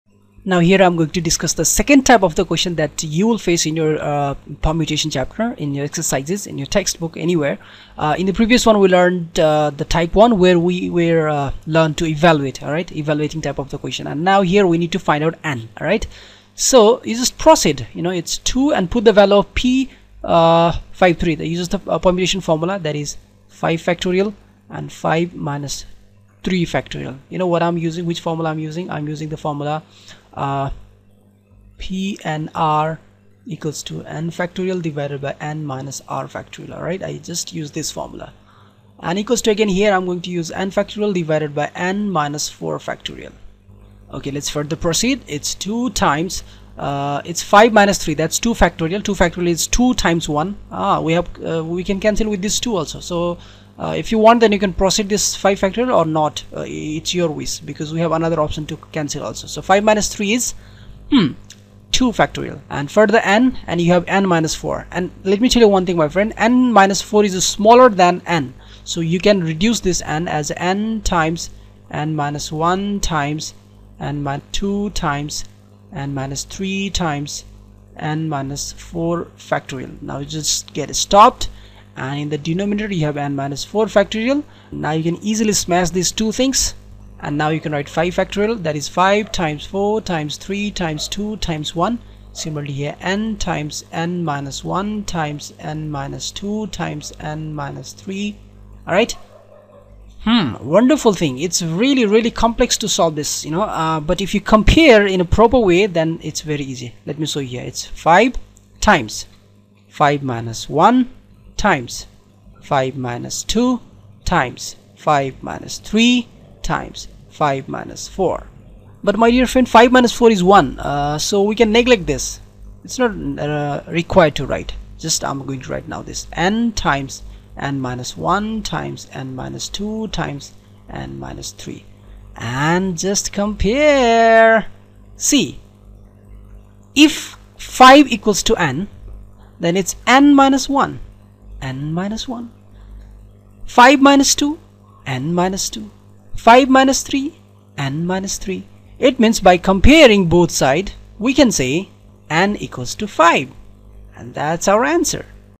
Permutations Questions - Finding Value of ' n' (Type 2) - Combinations & Permutations Lecture 25
Learn Permutations Questions - This Permutation & combination Lecture will teach you to find the value of n from a equation of permutations. This is the second type of the question generally asked in Permutations Mathematics, where we need to find the total number of items available for the arrangements.
To solve this Permutation question, we should first know the formula -

P(n,r) = n !/ (n-r) !

In above Permutations & Combination Problems , we have such expression both sides of the equation so , simultaneously we have to solve both sides, opening the expression by using formula.
Later few things will get cancelled and easily you will get the value of n. I believe this tutorial will gives you some idea to solve such similar types of dozens of Permutations & Combinations Problems.

If you any further doubt or any question on above Permutations & Combination video Lecture please let us know , in comment section below :)